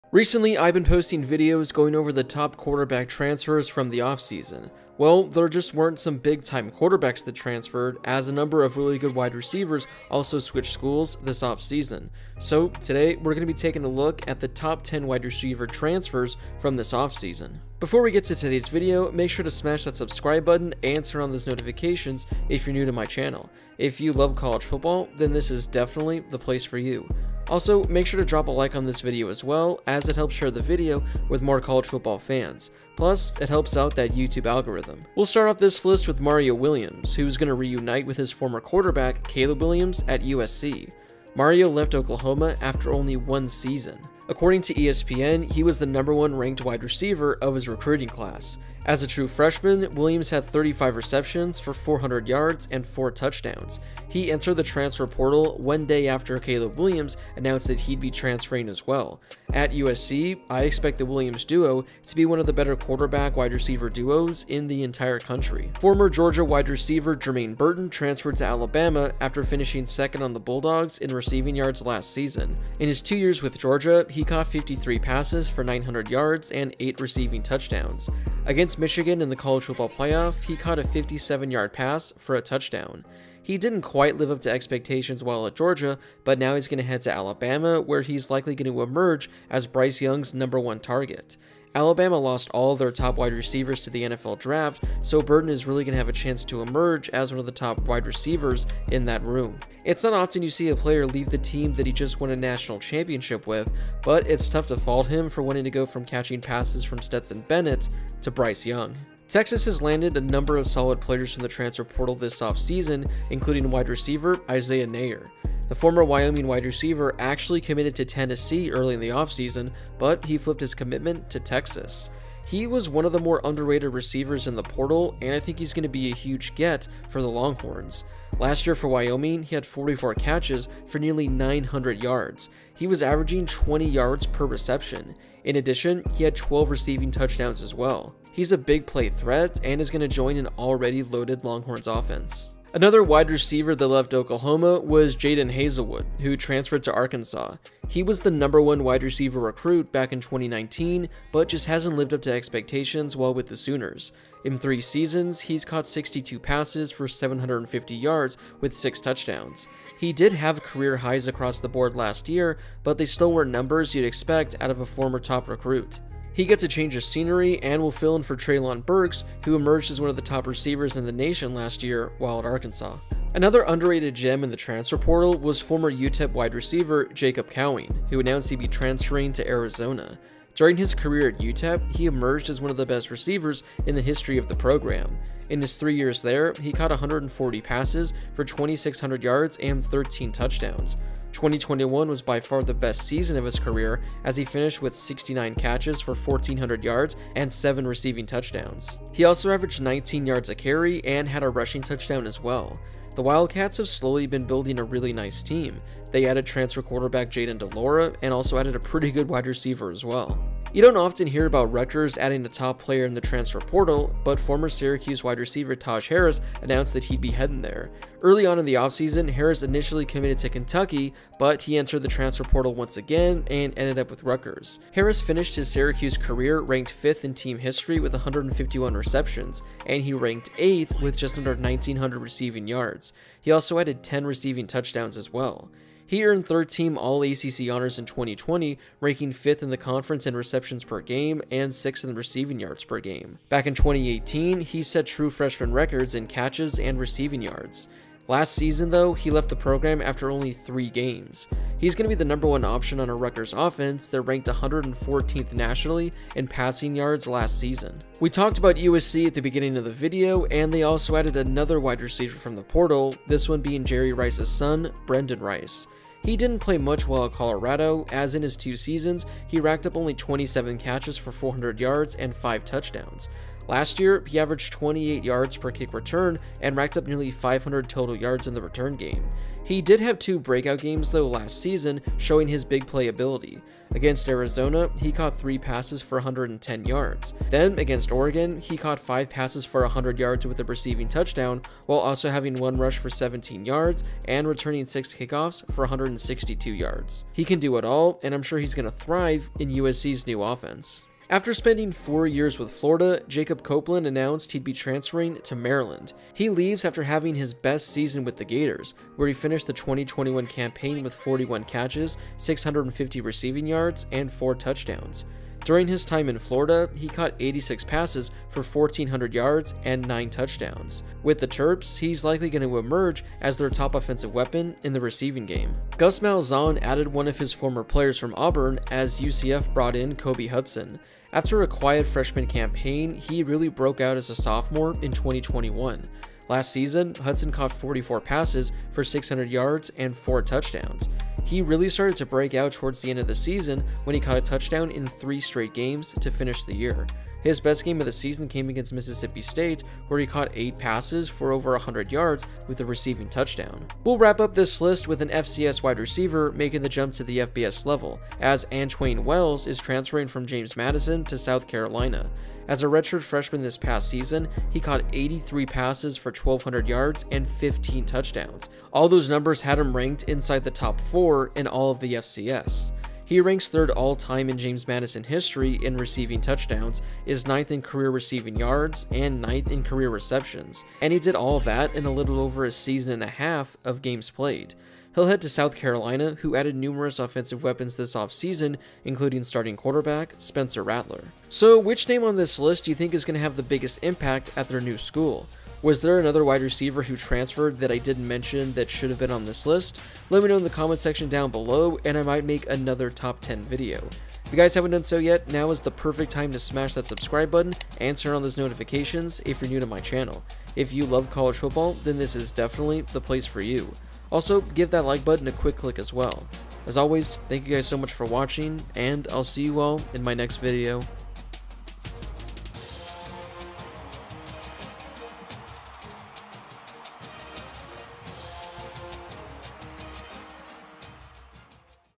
Top 10 Wide Receivers WHO TRANSFERRED in 2022
There were a number of great wide receivers who switched schools this offseason. Here are 10 who will make a huge impact at their new school.

If you love college football, this is the channel for you.